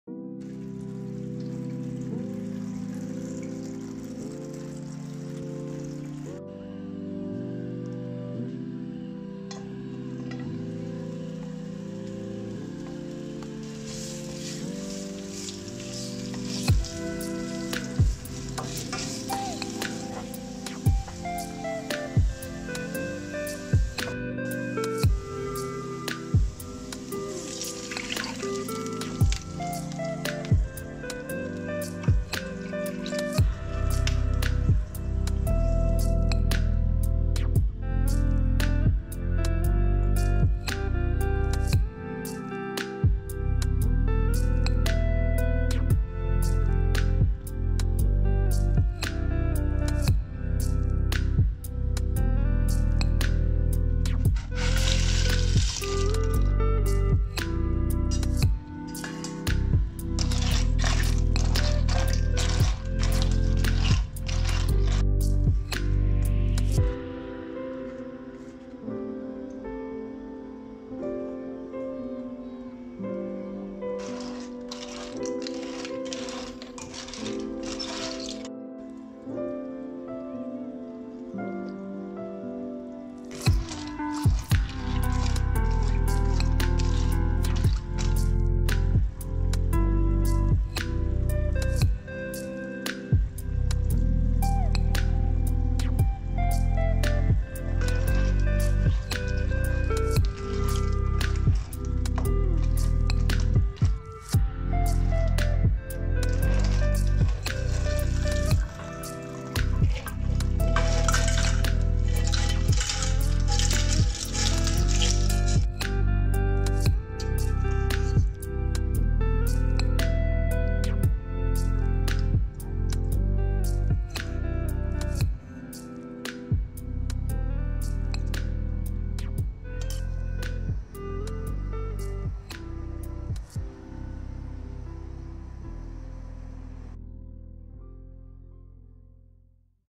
家常菜 #01，在家也能煮出好吃的上汤啦啦？？！！ “Homemade Clams In Superior Soup"
各施各法，这是我个人的食谱，
大家可以试一试在家煮。